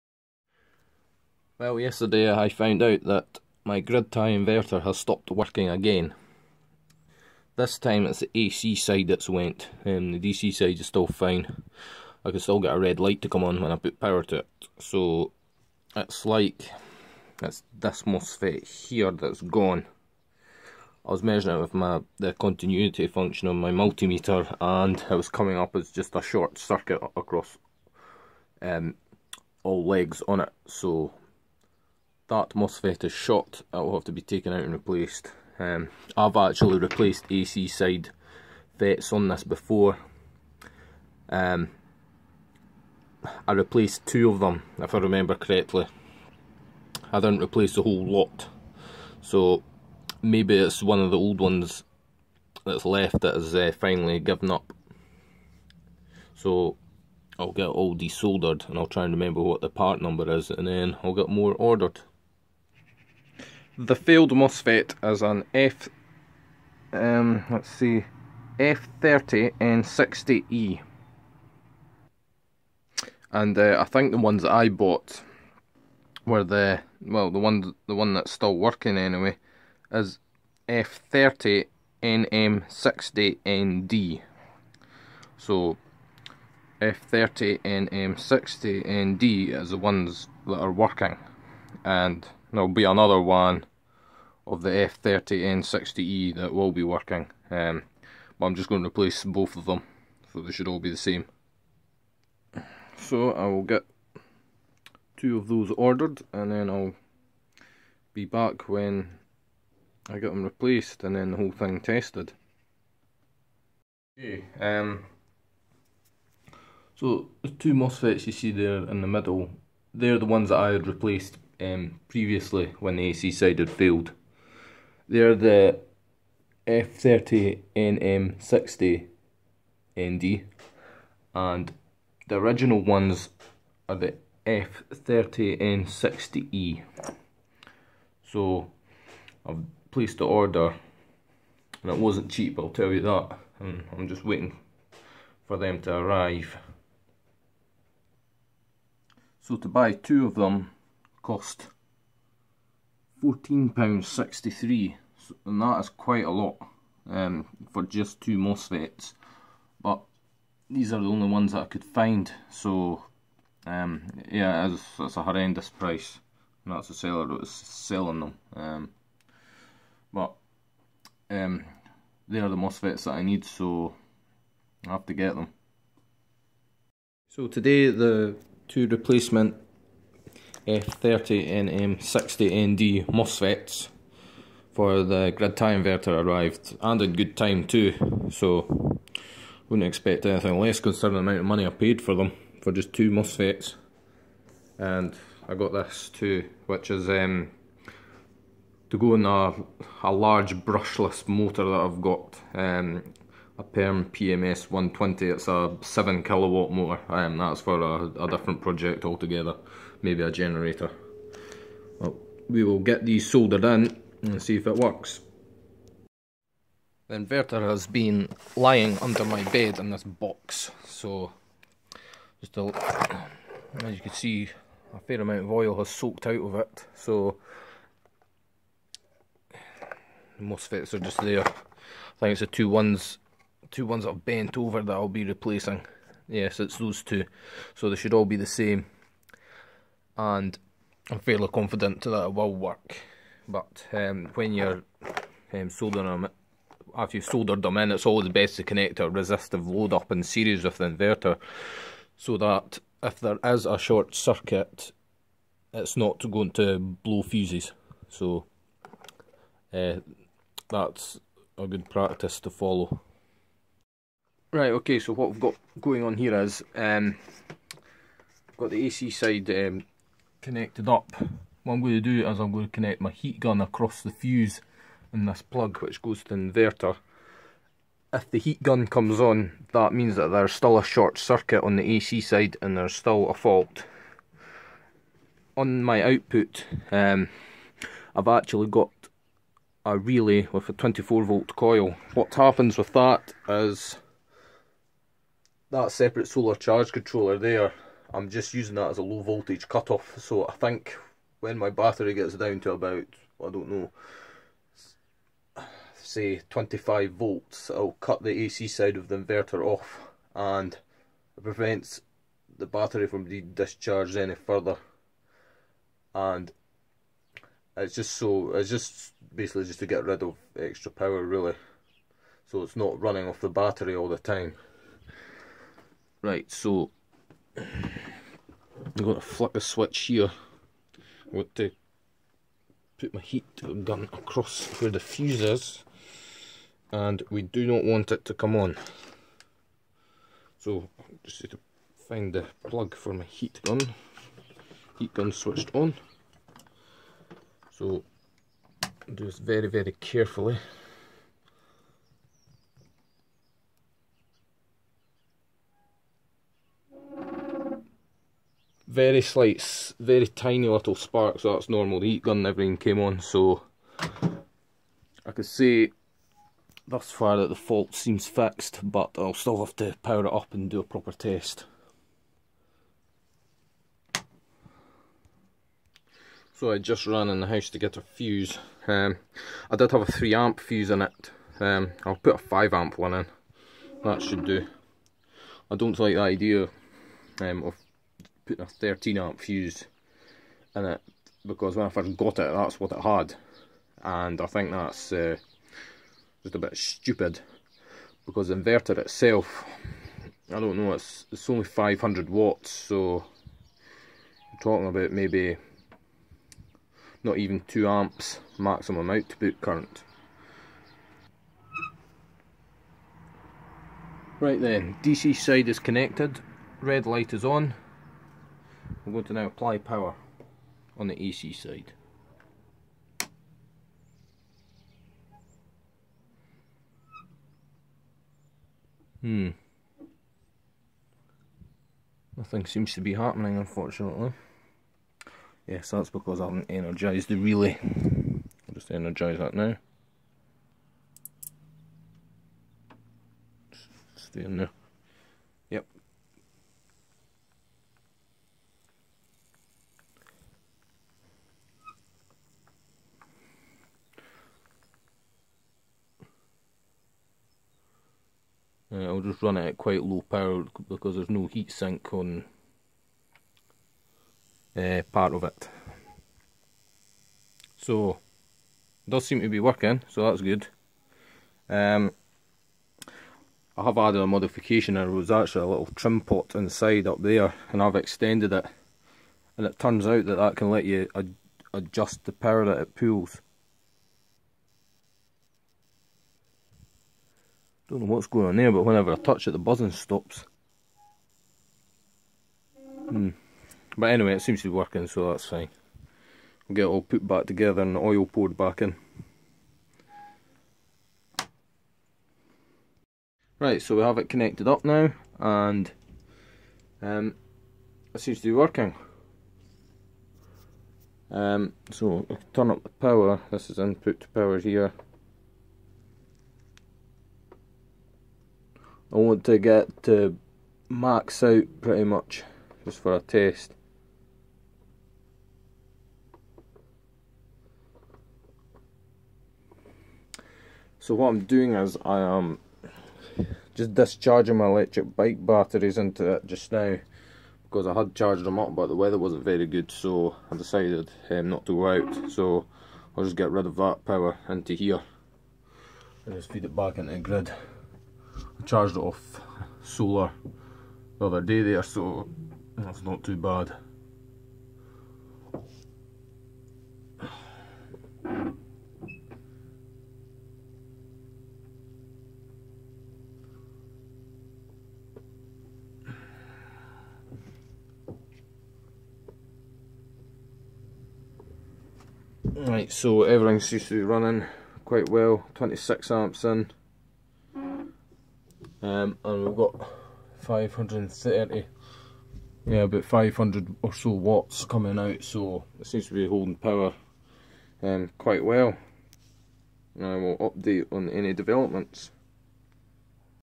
500 Watt Grid Tie Failure
The inverter broke again but I fixed it.